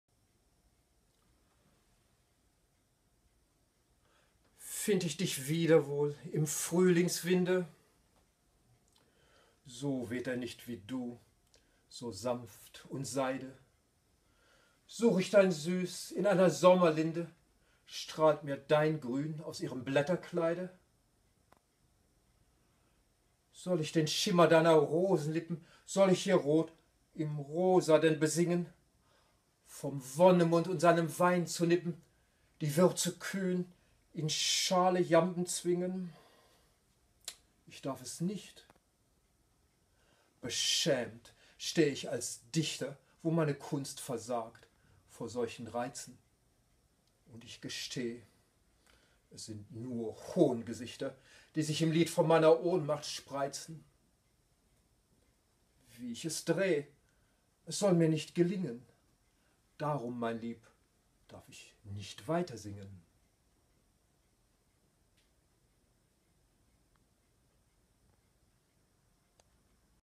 Find ich dich wieder wohl im Frühlingswinde? (Sonett von Rolf-Peter Wille)
Sonett Gedicht ( Shakespeare Parodie - Shall I Compare Thee to a Summer's Dream ) von Rolf-Peter Wille, rezitiert und gelesen November 2019, Taipeh.

Find ich dich wieder wohl im Frühlingswinde?
So weht er nicht, wie du, so sanft und Seide!
Such ich dein Süß in einer Sommerlinde?
Strahlt mir dein Grün aus ihrem Blätterkleide?

Soll ich den Schimmer deiner Rosenlippen,
Soll ich ihr Rot im Rosa denn besingen,
Vom Wonnemund und seinem Wein zu nippen,
die Würze kühn in schale Jamben zwingen?

Ich darf es nicht! Beschämt steh ich als Dichter,
Wo meine Kunst versagt vor solchen Reizen,
Und ich gesteh: Es sind nur Hohngesichter,
Die sich im Lied von meiner Ohnmacht spreizen.

Wie ich es dreh’, es soll mir nicht gelingen.
Darum, mein Lieb, darf ich nicht weitersingen.